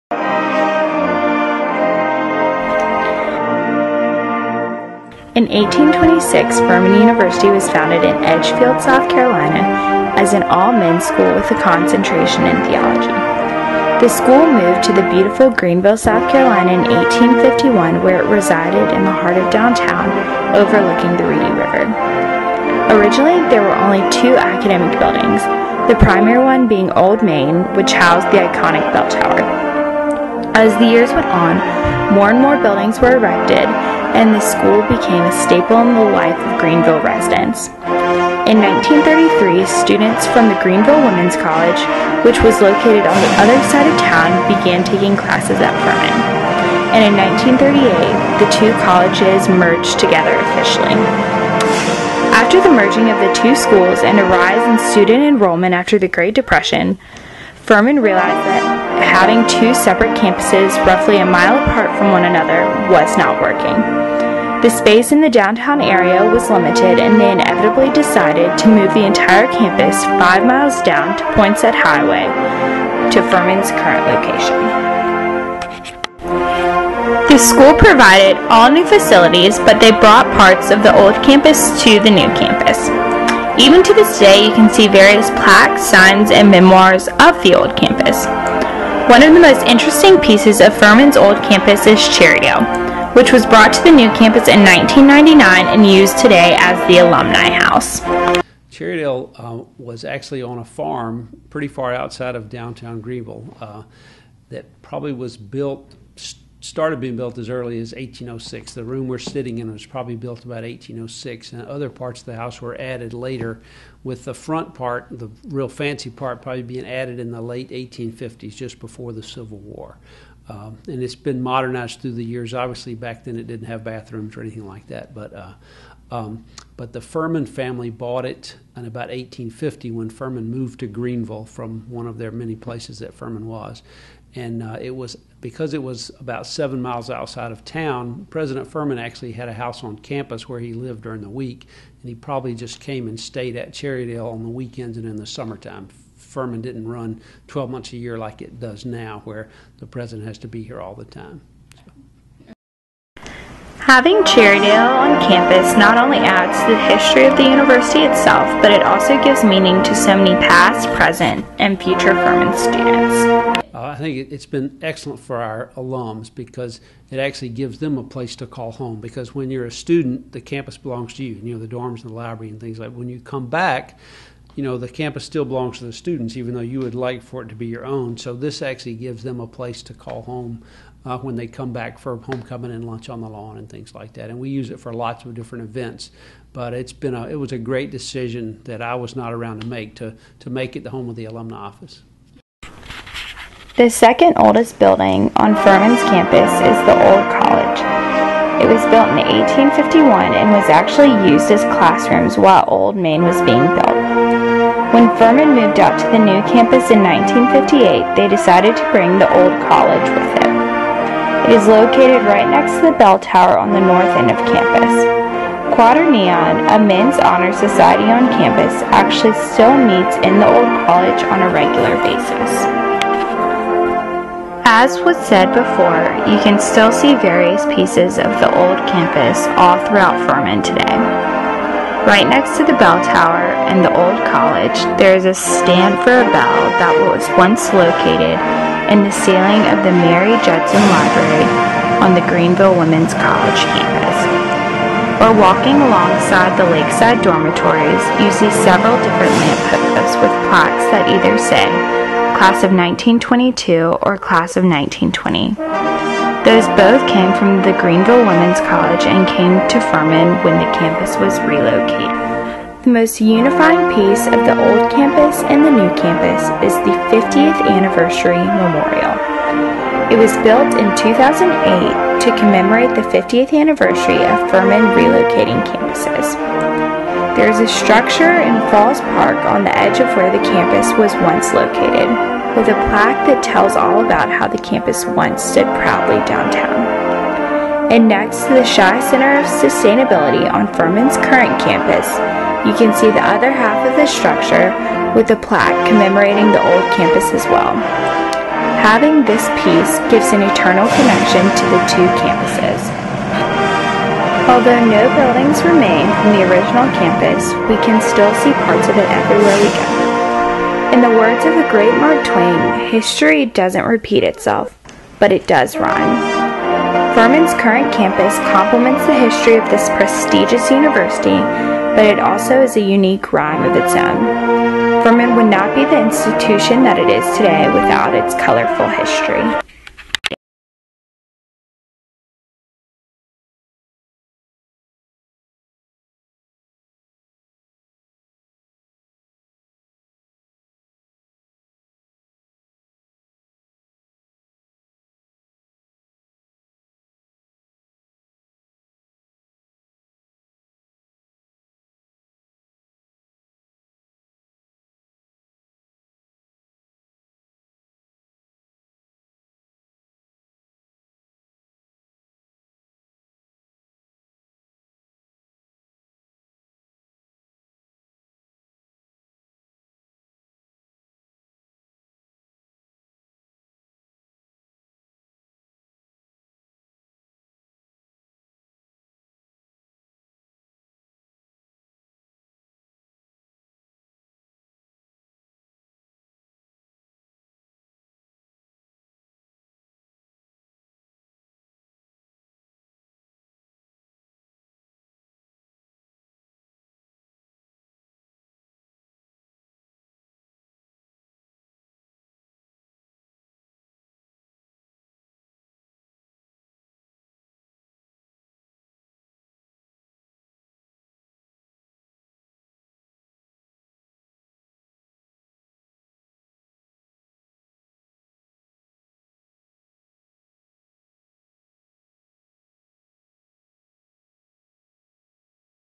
Furman University: Past and Present
A short film about Furman University's old a new campuses.